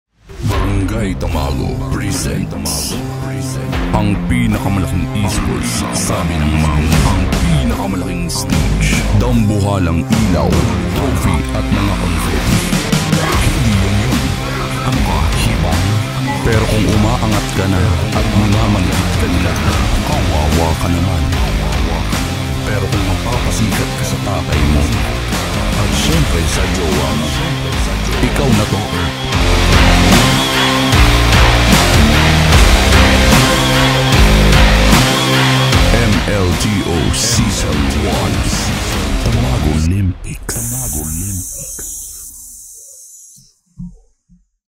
MLTO SEASON 1 : TAMAGOLYMPICS TRAILER
Bumuo ng team at magregister!

Deadline of registration: Feb 12, 2023
Start ng Balagbagan: Feb 18,2023 | Time 8 PM

Max of 20 teams para sa Season 1.

Prizepool: start from 3,000 php upto 10,000 php depende sa dami ng team na sasali. Kaya sali na!